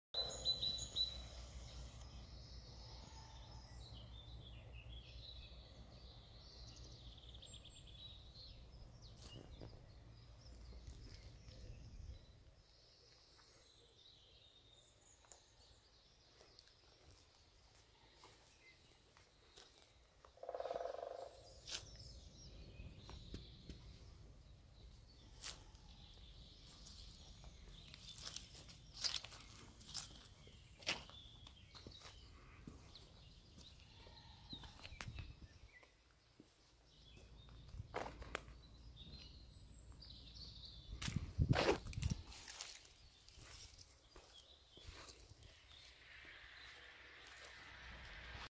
Boshuis Oosterbeek Veluwe
Dit is een opname van het Boshuis Oosterbeek - Arnhem dat gehuurd kan worden als vakantiewoning of als tijdelijke woonruimte (Short Stay). Kijk op